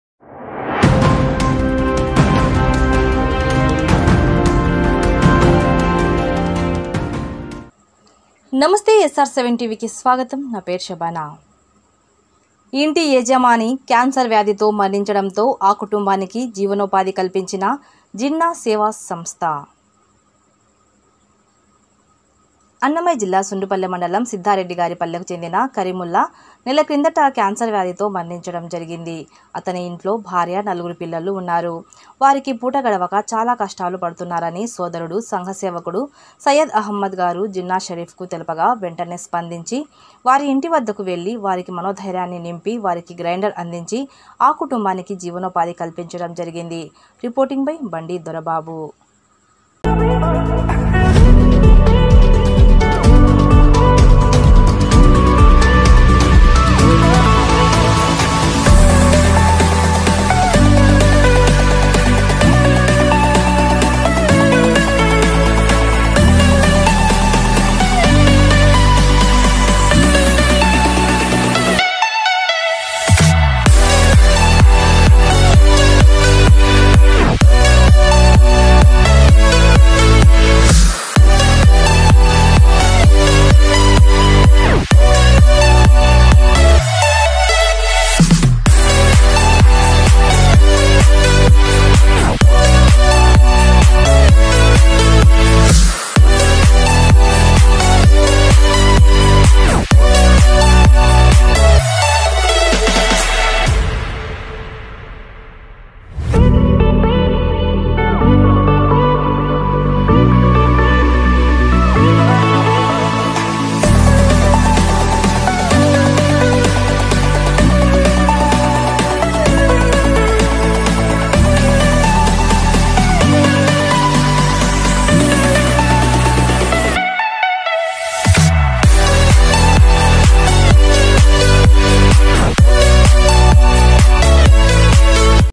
ఇంటి యజమాని క్యాన్సర్ వ్యాధితో మరణించడంతో ఆ కుటుంబానికి జీవనోపాధి కల్పించిన జిన్నా సేవా సంస్థ...
అన్నమయ్య జిల్లా సుండుపల్లె మండలం సిద్ధారెడ్డి గారి పల్లెకు చెందిన కరీముల్లా నెల క్రిందట క్యాన్సర్ వ్యాధితో మరణించడం జరిగింది, అతని ఇంట్లో భార్య నలుగురు పిల్లలు ఉన్నారు వారికి పూట గడవక చాలా కష్టాలు పడుతున్నారని సోదరుడు సంఘ సేవకుడు సయ్యద్ అహ్మద్ గారు జిన్నా షరీఫ్ కు తెలుపగా వెంటనే స్పందించి వారి ఇంటి వద్దకు వెళ్లి వారికి మనోధైర్యాన్ని నింపి వారికి గ్రైండర్ అందించి ఆ కుటుంబానికి జీవనోపాధి కల్పించడం జరిగినది.

రిపోర్టింగ్ బై బండి దొరబాబు